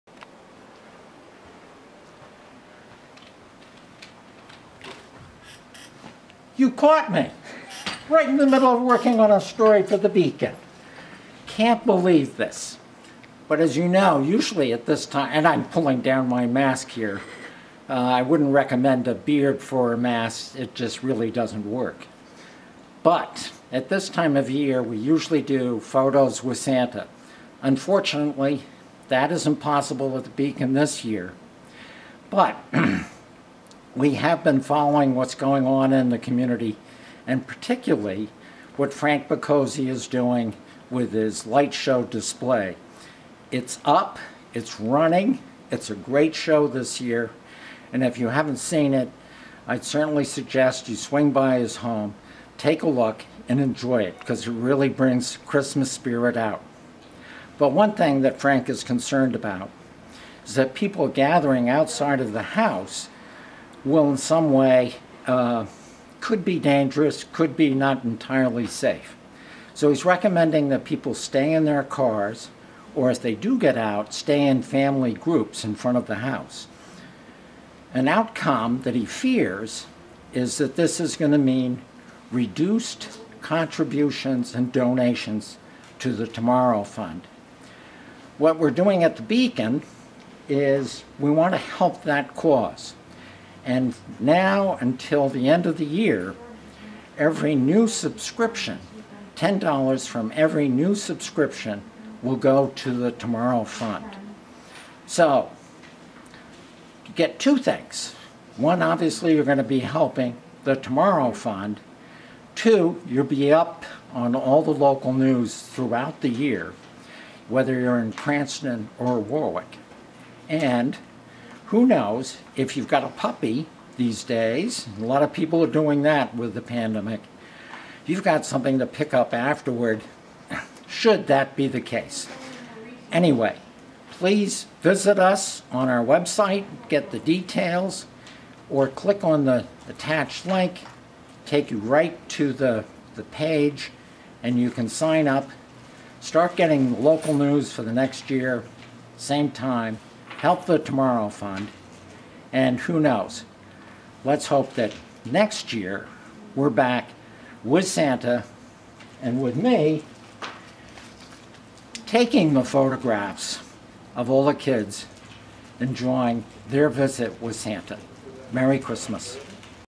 Beacon | Herald - Lights For Children With Cancer 2020
Love the lights of the holiday season?  The Warwick Beacon | Cranston Herald knew there were concerns that the donations received at The Picozzi Family Christmas Display might be in jeopardy this year so they spoke to Santa and he sent a helper to share how YOU can help, be up on what's happening in your community and even have something to repurpose for that new puppy - all year long!

You can help to make a difference.  The Tomorrow Fund provides support to children with cancer and their families treated at Hasbro Children’s Hospital in Providence, RI. The Warwick Beacon and Cranston Herald will donate $10 of every new subscription ordered above to The Tomorrow Fund.